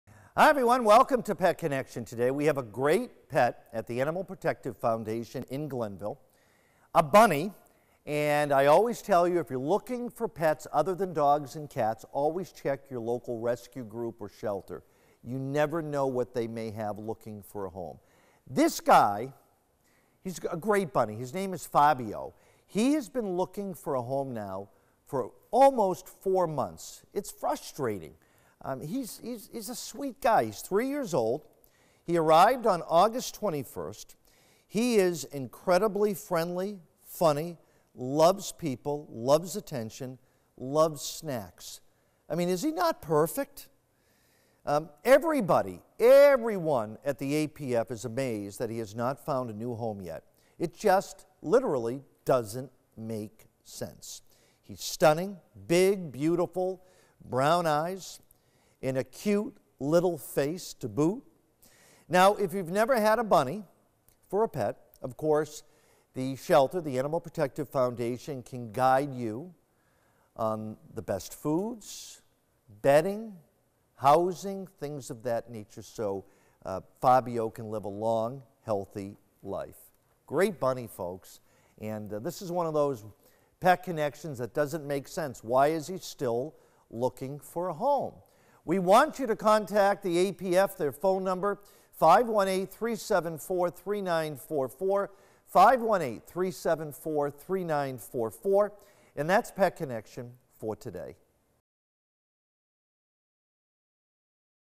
Pet Connection Fabio 12-8-21
Who needs Easter to like Bunnies.
You've got to stop by and meet "Fabio" ...he is the sweetest and most friendly bunny you will ever meet !

Pet Connection 12-8-21

We need to find a home for this guy. been too long.

Fabio is about 3 years old and has been with us since August 21st !He is And incredibly friendly funny he loves people he loves attention he loves snacks.  We are all amazed that he has not found a new home yet. It just doesn't make sense.

He is stunning. big beautiful brown ears. and a cuter littler face.

If you have never had a bunny-of course the shelter can help you out.